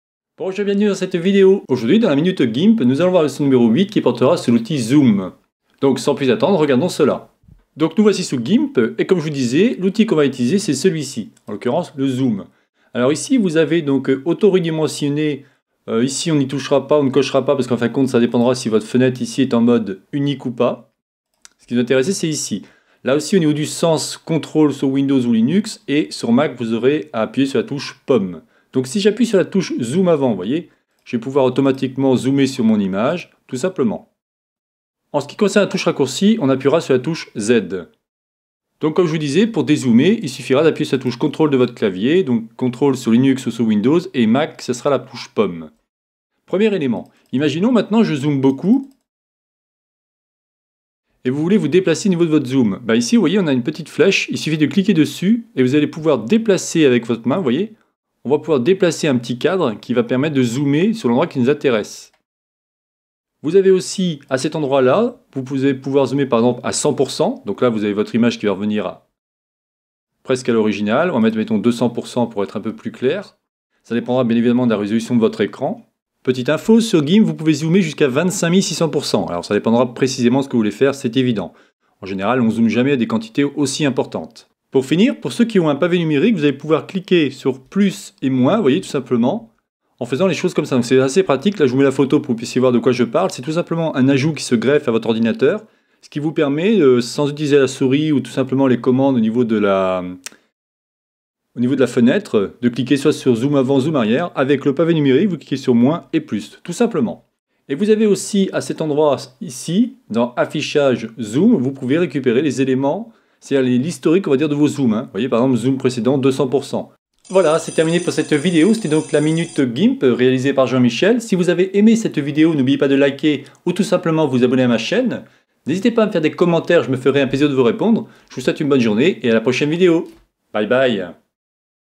Gimp - 08 - Zoom
Dans cette vidéo, nous allons apprendre à utiliser l'outil Zoom sur gimp avec ses différentes fonctions, ainsi que ses différentes techniques de zoom.

Plus d’infos:

Lien google plus - Hashtags Gimp